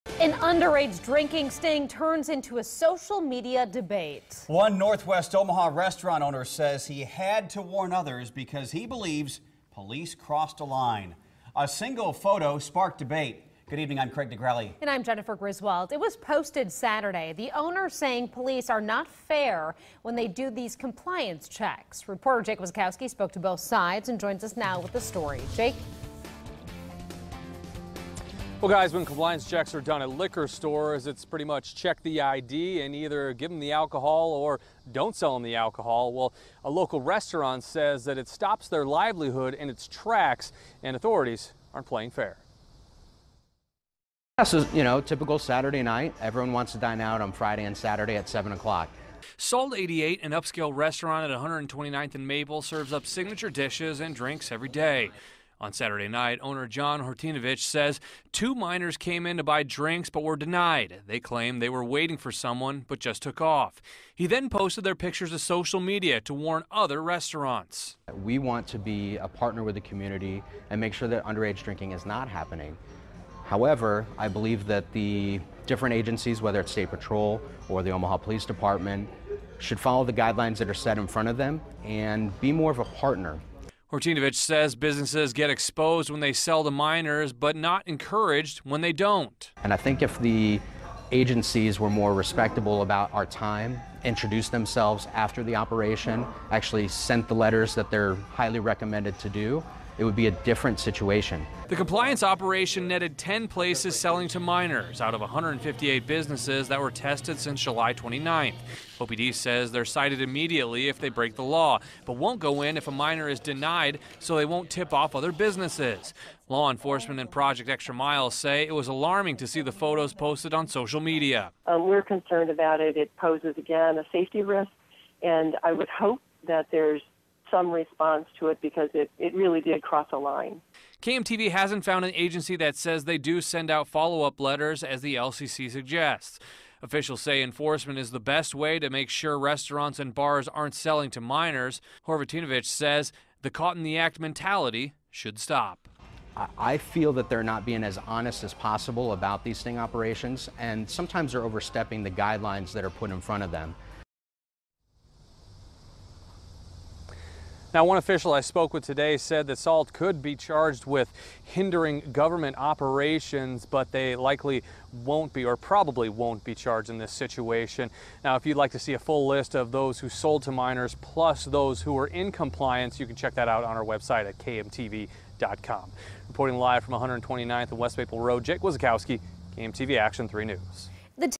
Restaurant warns of alcohol compliance check
OPD caught a handful of businesses selling alcohol to minors in a two week period, but one restaurant warned others when they refused to sell to the minors.